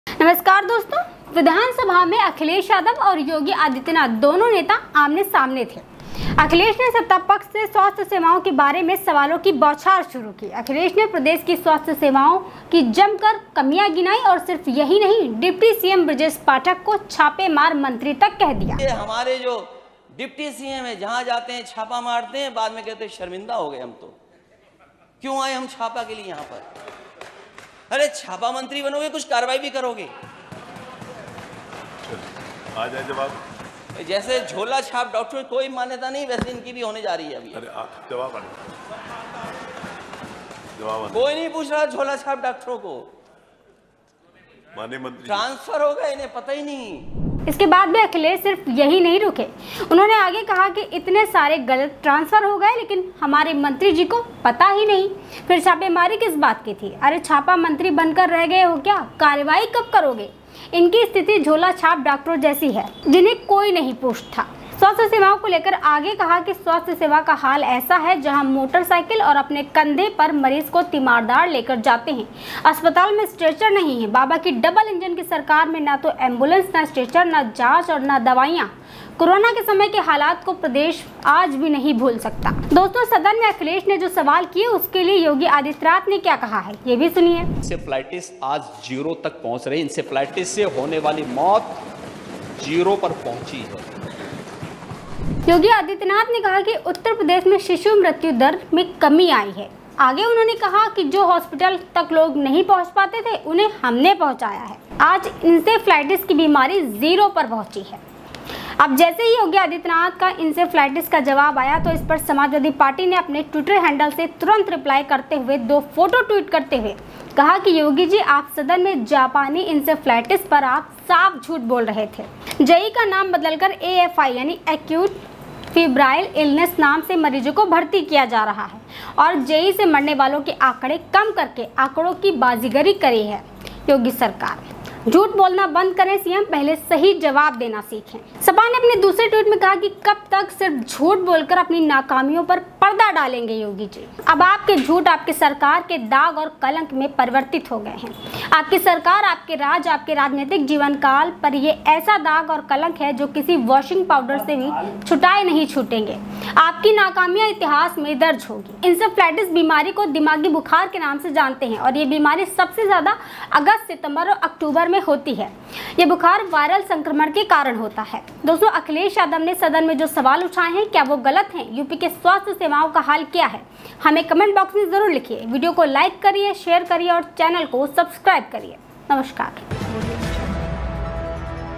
सदन में भिड़े Akhilesh और Brajesh Pathak | up Vidhansabha
Akhilesh  ने Brajesh Pathak की उड़ा दी धज्जिया | up Vidhandabha | Brajesh Pathak | Akhilesh
नमस्कार दोस्तों विधानसभा में अखिलेश यादव ( Akhilesh Yadav ) और योगी आदित्यनाथ (  Yogi Adityanath ) दोनों नेता आमने सामने थे..अखिलेश ने सत्ता पक्ष से स्वास्थय सेवाओं के बारे में सवालों बारे में की बौछार करनी शुरु की..अखिलेश ने प्रदेश की स्वास्थ्य व्यवस्था की जमकर कमियां गिनाईं और सिर्फ यही नहीं डिप्टी सीएम ब्रजेश पाठक को छापेमार मंत्री तक कह दिया.